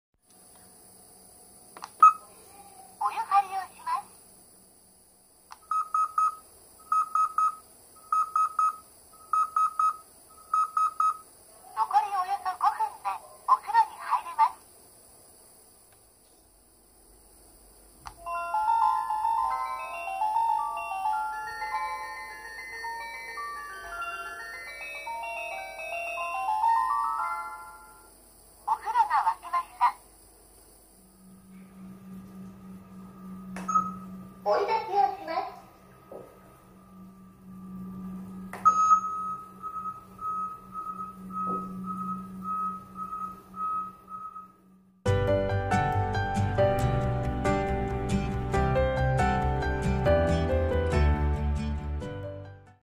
リンナイ給湯器☆お風呂が沸きました♬
リンナイ給湯器の音声集めてみました！